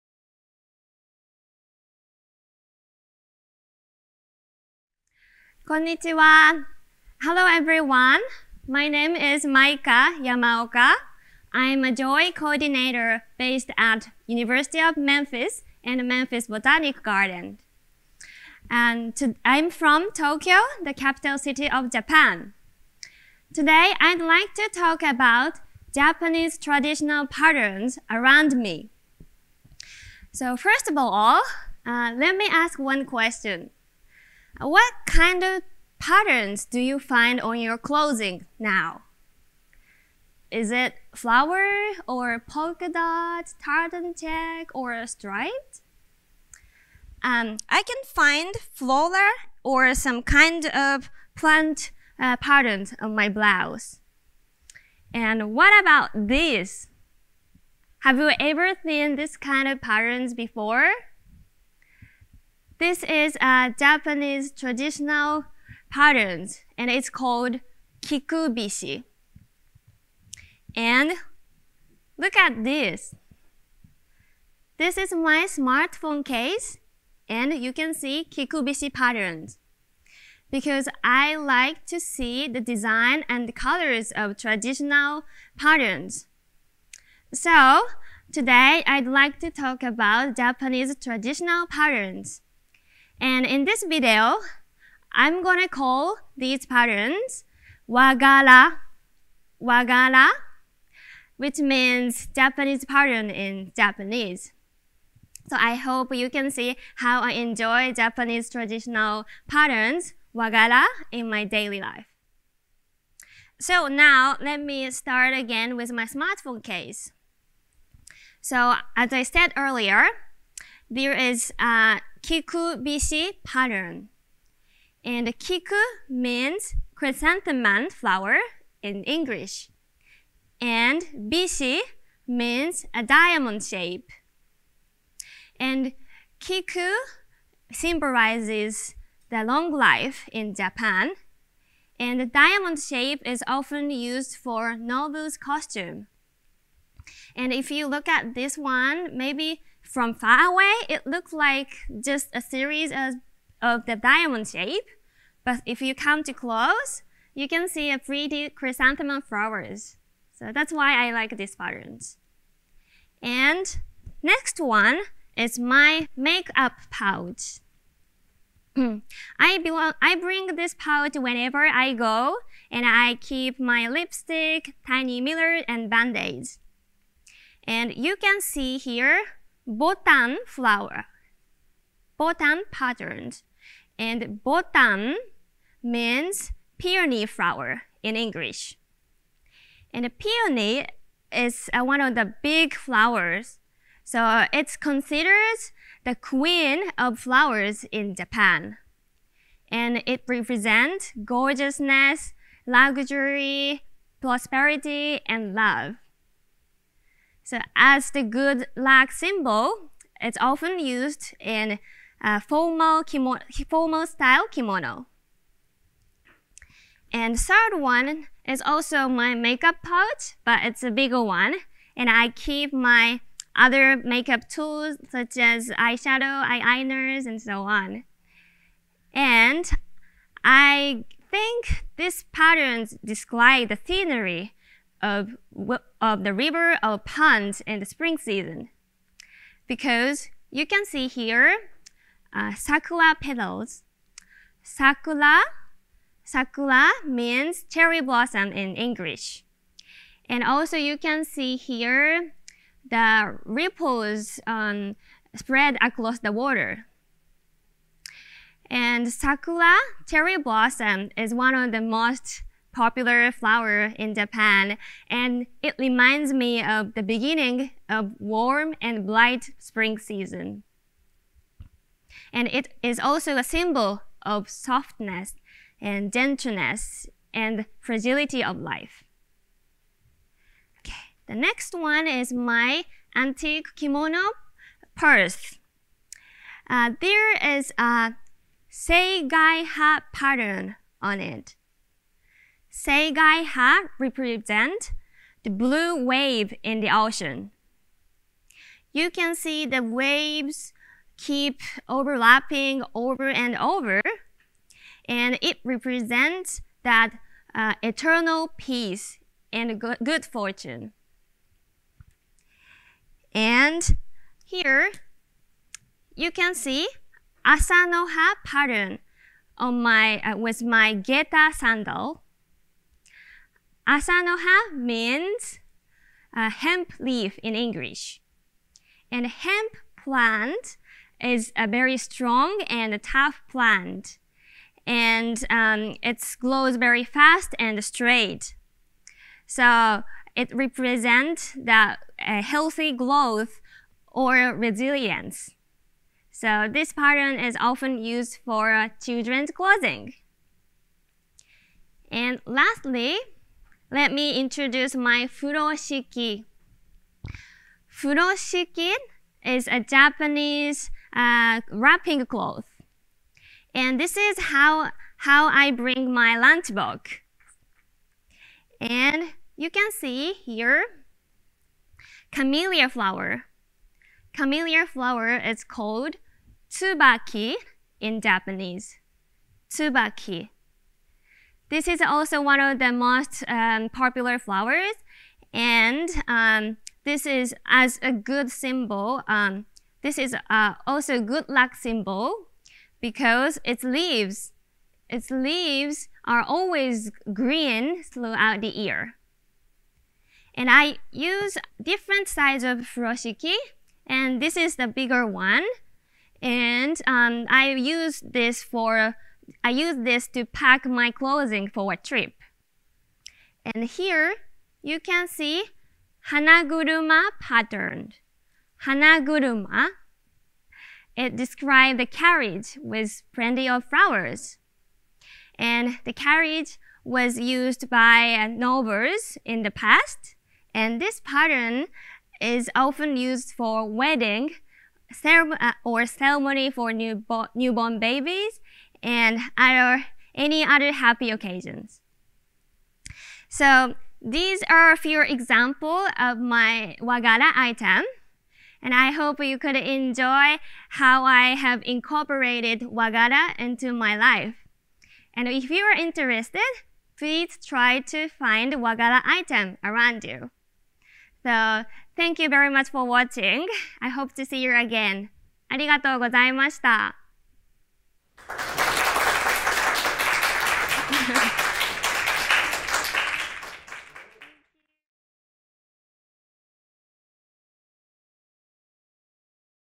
JOI 20th Anniversary: Cultural Festival | Traditional Patterns with Maika Yamaoka
As part of Japan Outreach Initiative's 20th Anniversary Celebrations, the Year 19 Coordinators held a virtual cultural festival presenting on unique and lesser-known aspects of Japanese culture. Maika Yamaoka of the University of Memphis and Memphis Botanic Garden, TN introduces some traditional patterns and textiles inspired by nature!

Follow JOI!